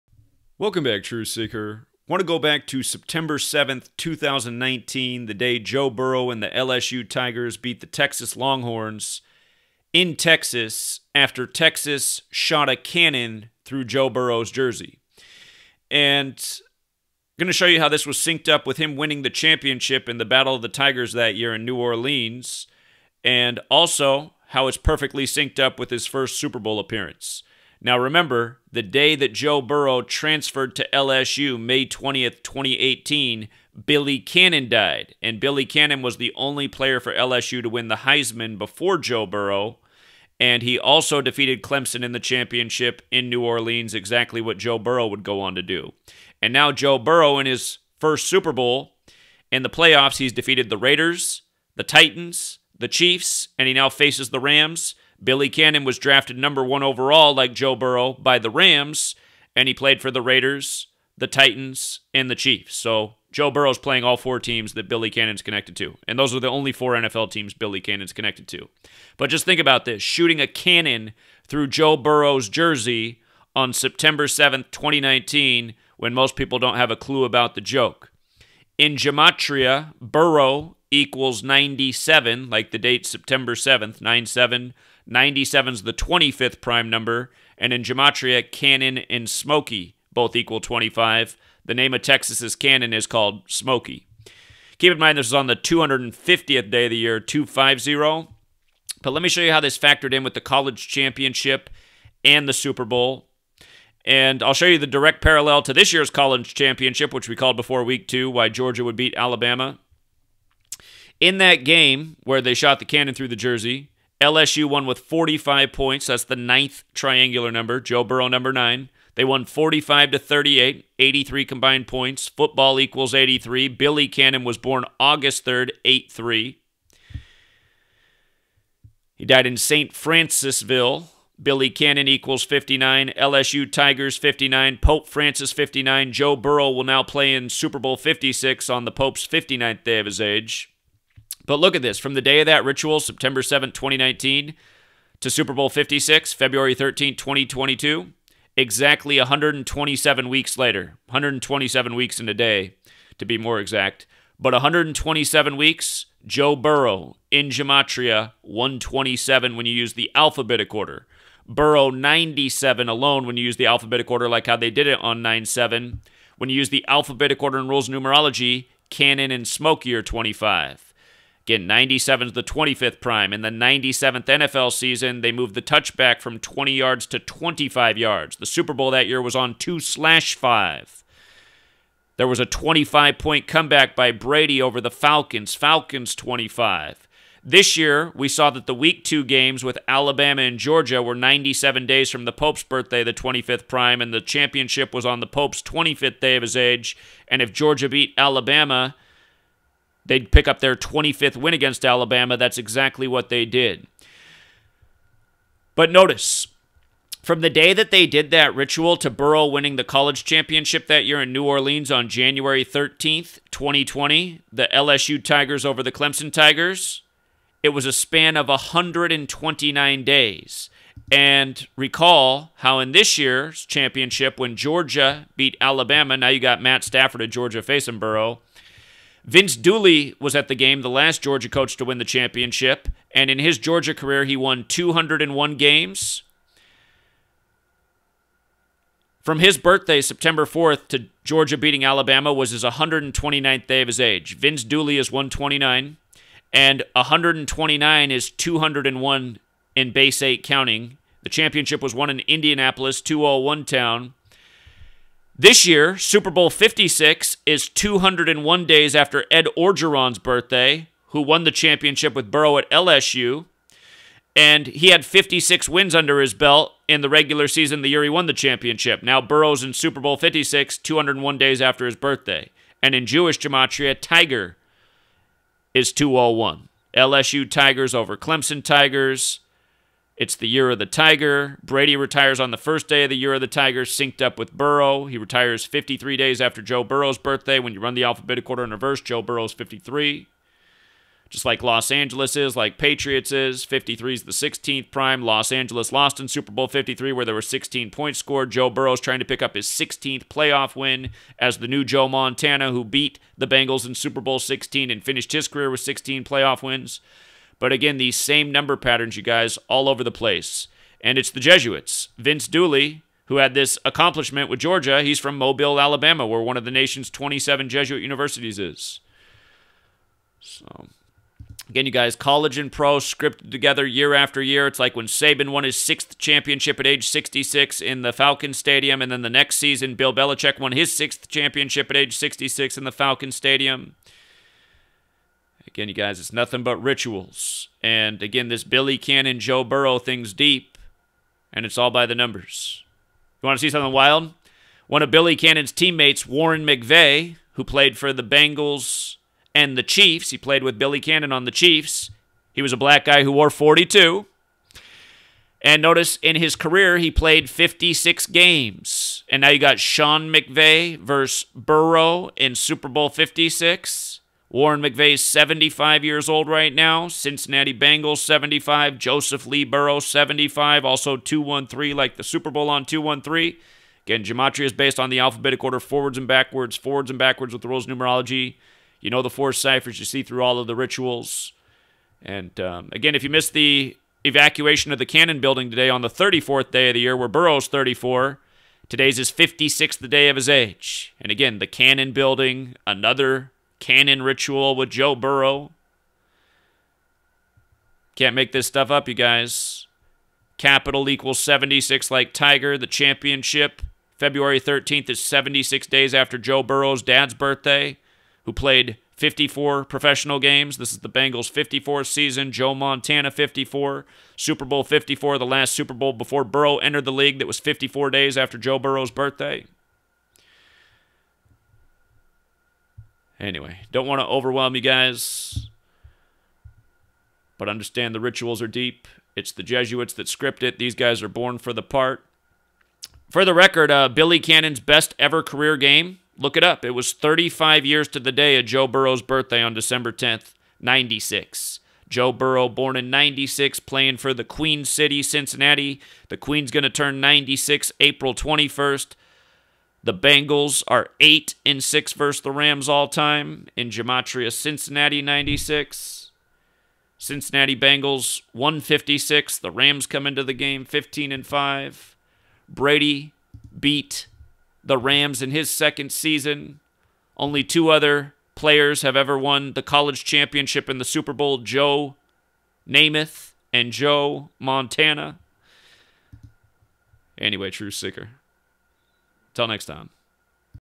How Texas shooting a cannon through Joe Burrow's jersey syncs with LSU championship & Super Bowl 56
To get your hoodie follow these 2 steps:
2) Send $50 for Large or XL or $60 for XXL (S & M sold out, sorry)